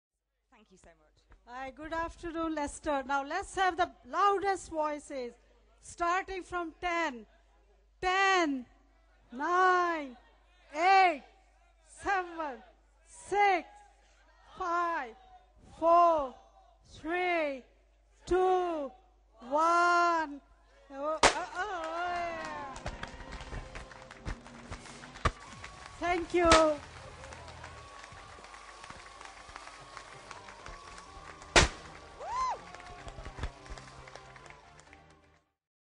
Countdown 365 Days to Go
Footage from the year to go event. Lord Mayor of Leicester, Cllr Manjula Sood